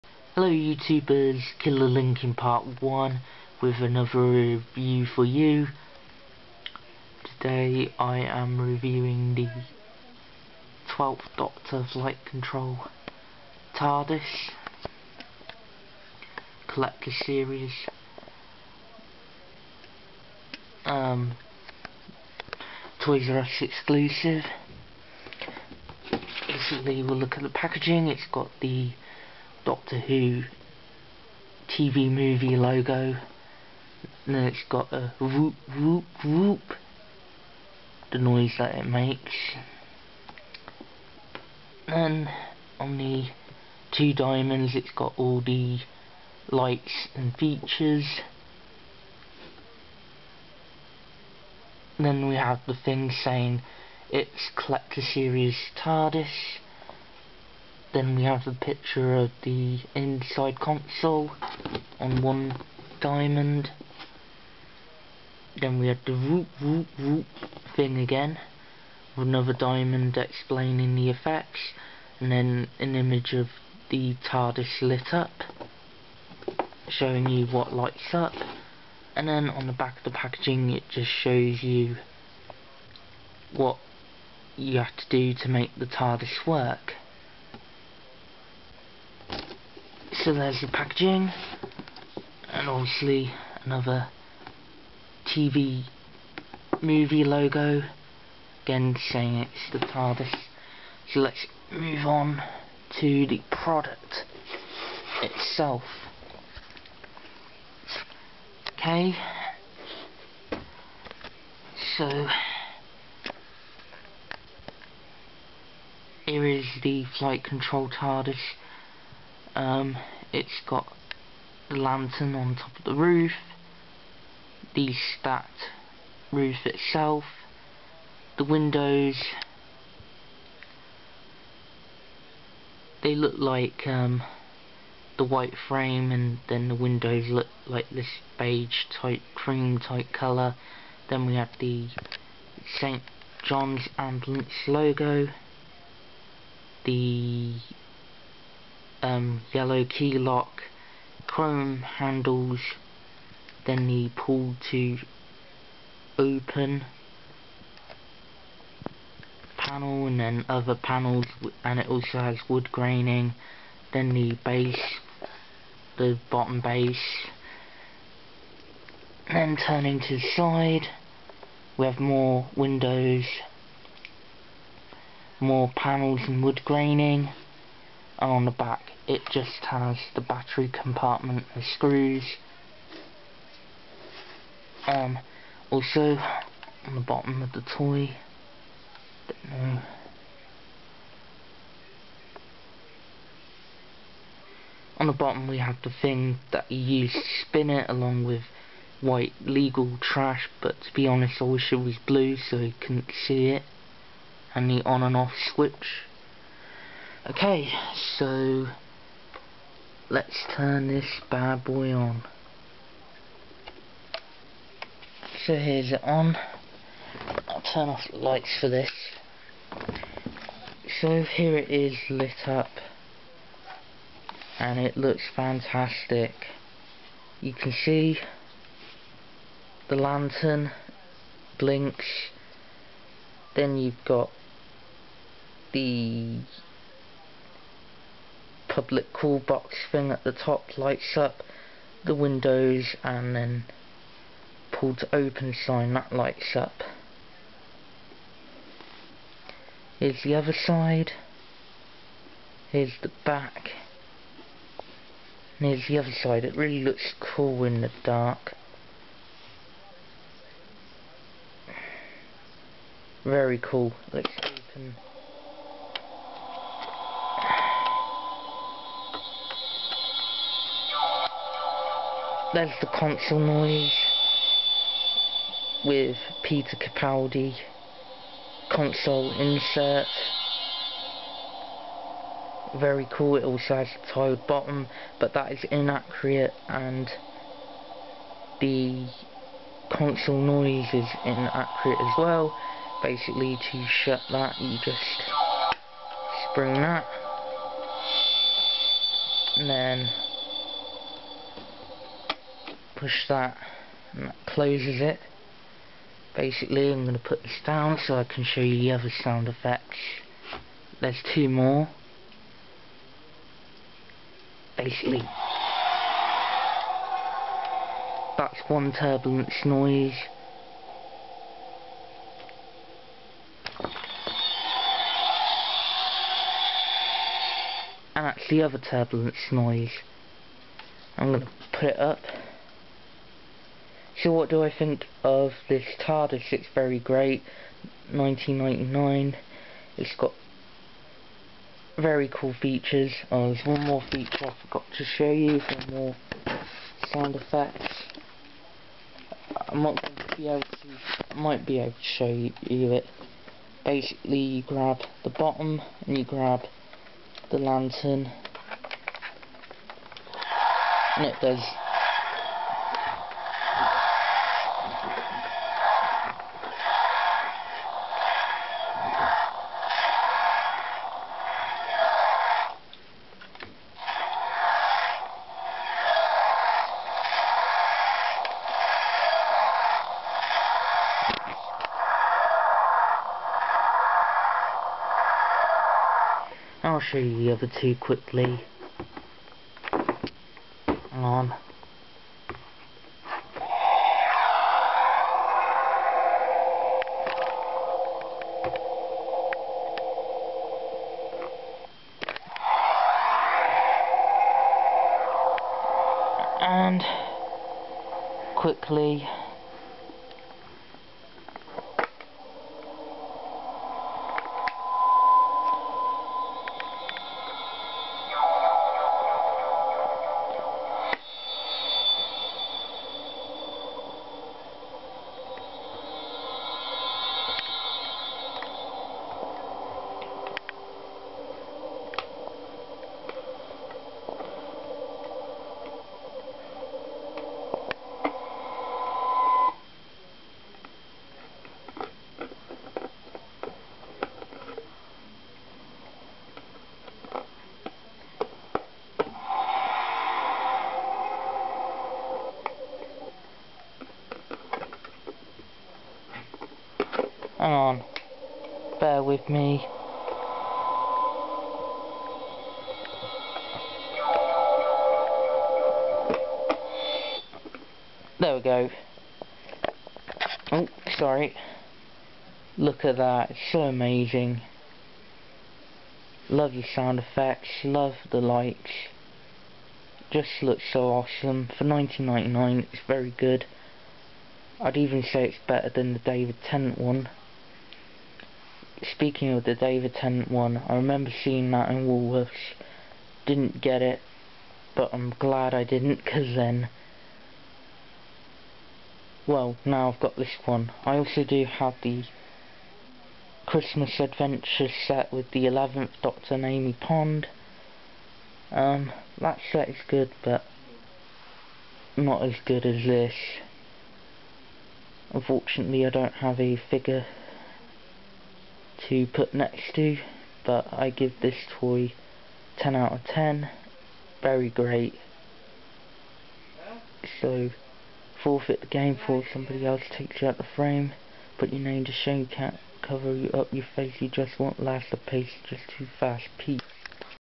Dr Who Toys R Us Flight Control Tardis Toy Review
today i take a look at the collector series toys r us exckluesive flight control tardis goes